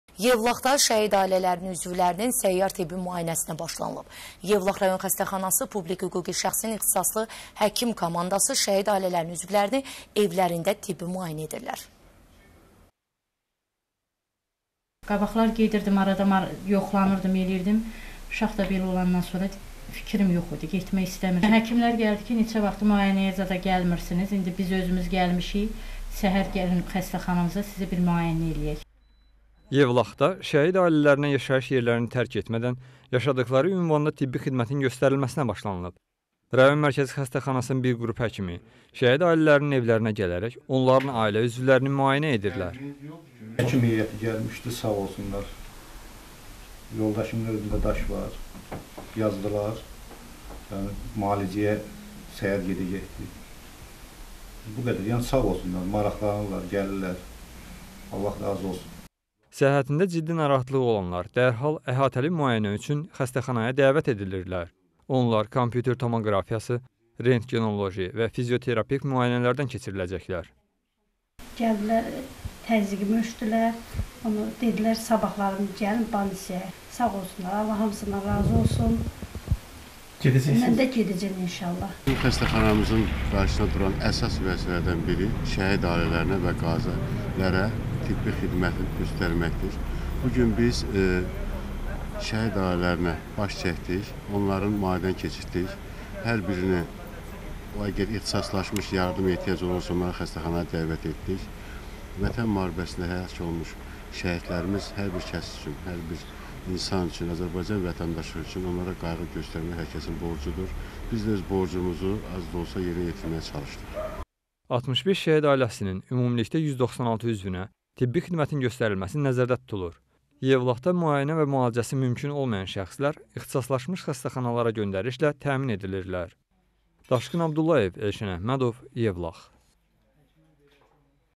ŞƏHİD AİLƏLƏRİ SƏYYARİ TİBBİ MÜAYİNƏ OLUNUR
Yevlaxda şəhid ailələrinin üzvlərinin tibbi müayinəsi onların yaşadıqları ünvanlarda aparılır.